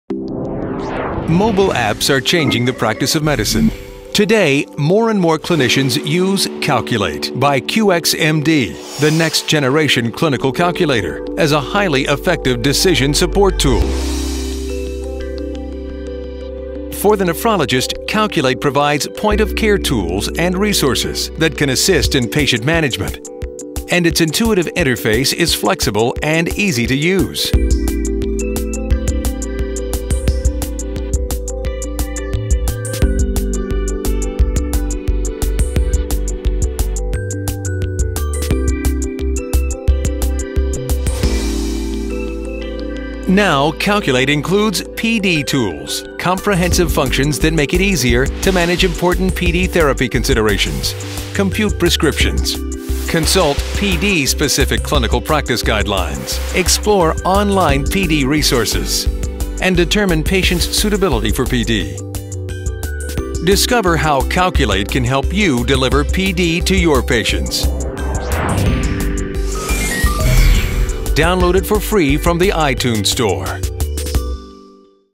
Peritoneal Dialysis Tools Embedded in Calculate by QxMD
Mobile app 'Calculate by QxMD' now includes a robust section to support peritoneal dialysis practice.

Content includes:

Access Care and Complications Manual including detailed content on management of PD catheters and infectious peritonitis

PD Prescription Guide with calculator to assist in determining the PD prescription and extensive manual to learn principles behind PD dose, adequacy and modelling

Rapid Access to Clinical Practice Guidelines including CSN PD Adequacy Guidelines and Recommendations 2011, ISPD PD-Related Infections Recommendations 2010 and ISPD Guidelines for Peritoneal Access

Quick Assessment of PD Candidacy using the MATCH-D tool